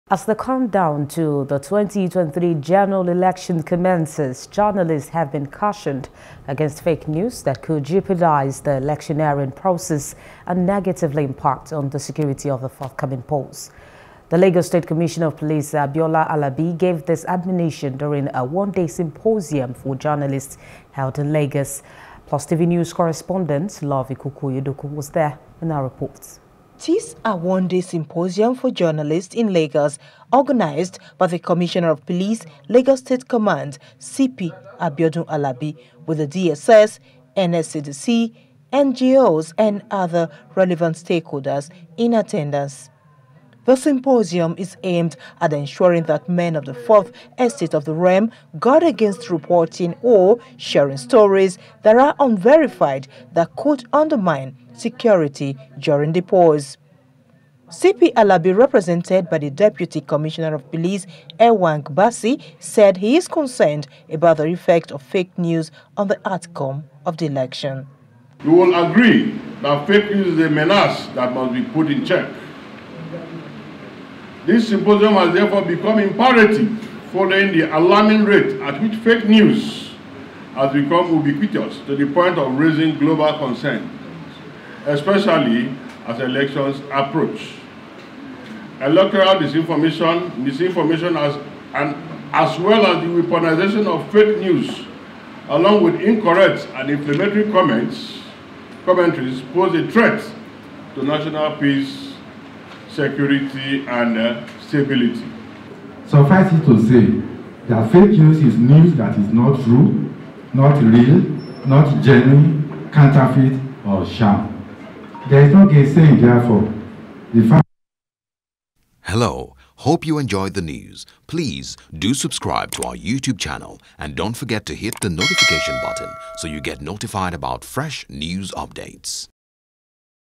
2023 Elections: Commissioner Of Police Warns Journalists Against Fake News
As the countdown to the 2023 general elections commences, Journalists have been cautioned against Fake News that could jeopardize the electioneering process and negatively impact on the security of the forthcoming polls.